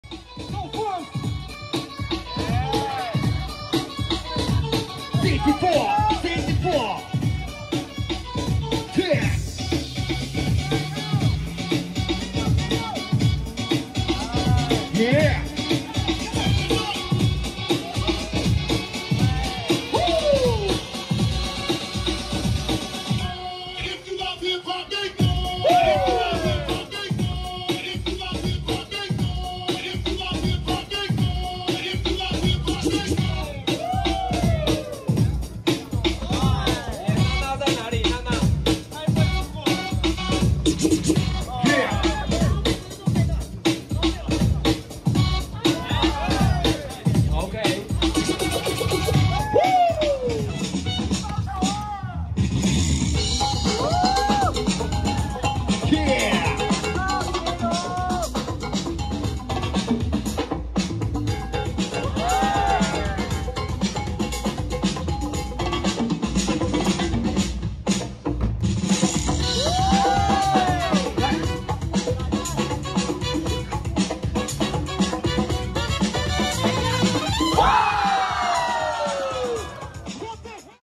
Bboy Qiru took the W at Freestyle Session Taiwan.
Rep: City 4

He took the 3 v 3 championship 🏆 with Bboy Shao and Bboy Wild Jun.